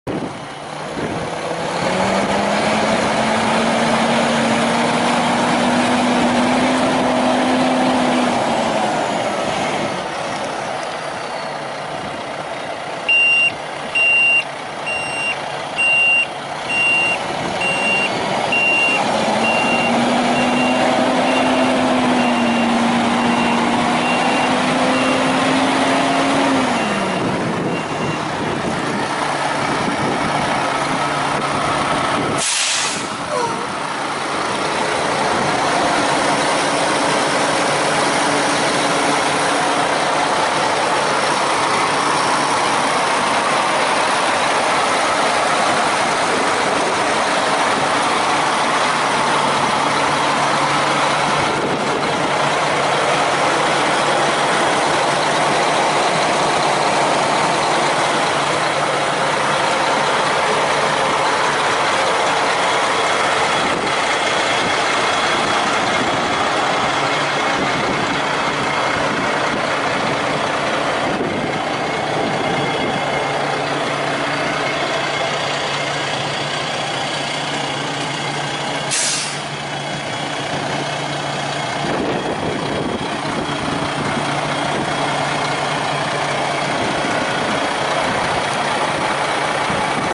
2014 INTERNATIONAL PROSTAR For Sale
Truck starts, runs, and drives.
Navistar 12.4L Engine.
10 Speed Transmission.
Professionally Tuned to 500hp.
Tires: 11R22.5 are all near new.
Air Sliding 5th Wheel.
Wheelbase: 188".
Cab to Axle: 115".
Cab to end of Frame: 168".
We take Trades.
We Finance.
More items from this seller.
Click 'Midwest Equipment Auctions' to the Right of the pictures to see more items we have selling.